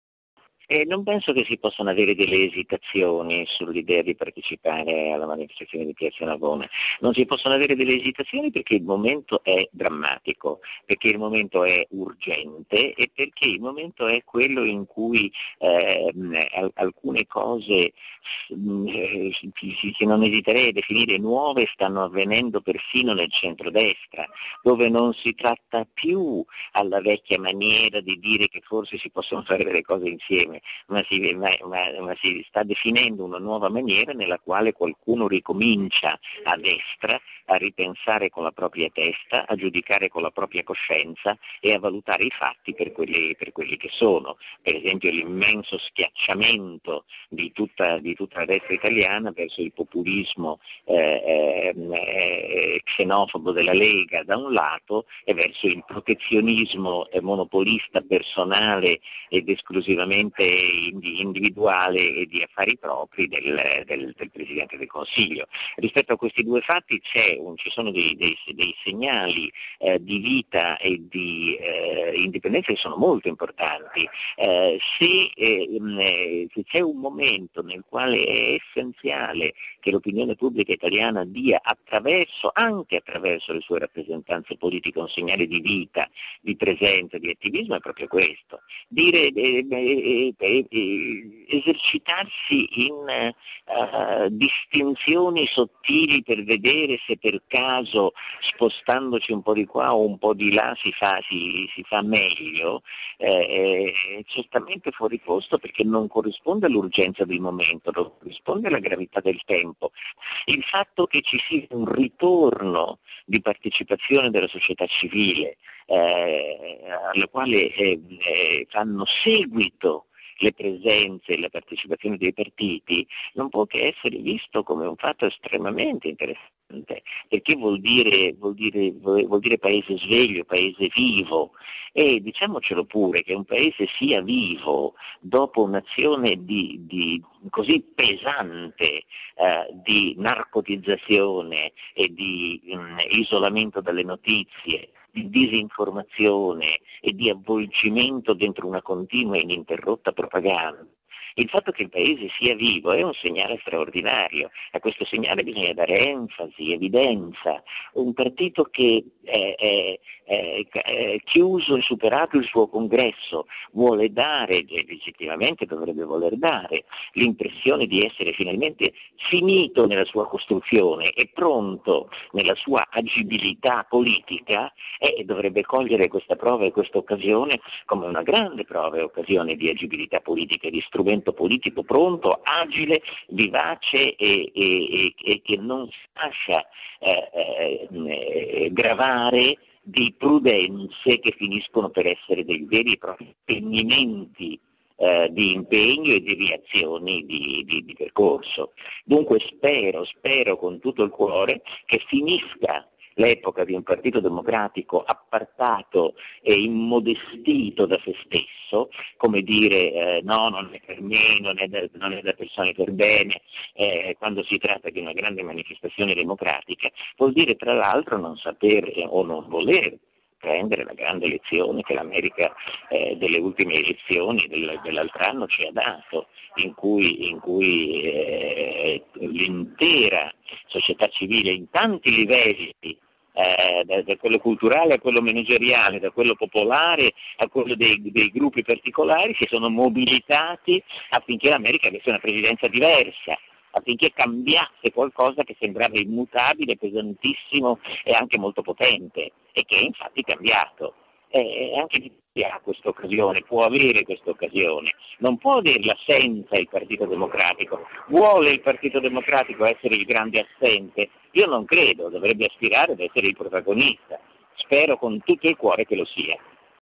"No B. Day", Furio Colombo a Bersani: "Momento drammatico, non si possono avere esitazioni"
"Il Partito democratico dovrebbe cogliere il segnale di vitalità che viene dalla società civile". L'invito di Furio Colombo al segretario del Partito democratico perchè aderisca alla manifestazione del 5 dicembre 2009, contro le leggi ad personam e per le dimissioni di Silvio Berlusconi.